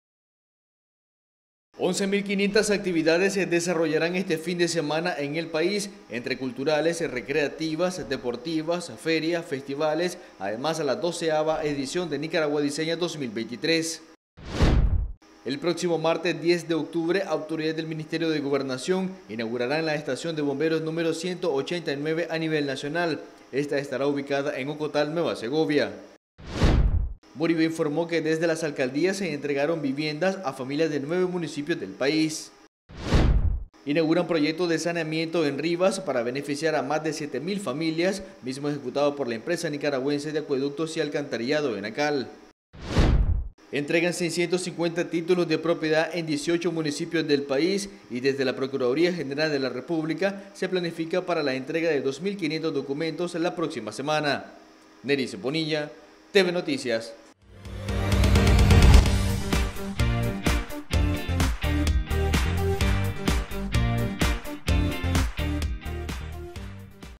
Más de 11 mil actividades a realizarse este fin de semana
Once mil 500 actividades se desarrollarán este fin de semana en el país, entre culturales, recreativas, deportivas, ferias, festivales; además la doceava edición de Nicaragua Diseña 2023.